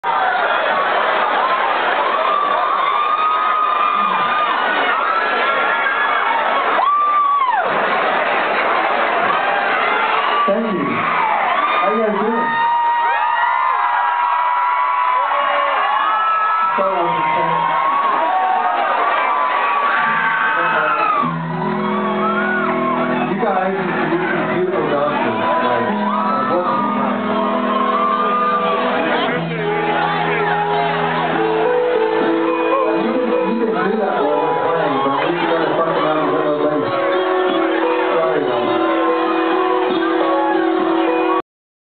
Gavin Rossdale & Chris Traynor BUSH setting up The Bank Bellagio 2/19/2011 Vegas
41 seconds, short video of Chris Traynor & Gavin Rossdale of Bush getting ready for their acoustic set at The Bank at Bellagio February 19, 2011

The crowd was very loud and so the sound is not the greatest. Also I don't really have a steady hand.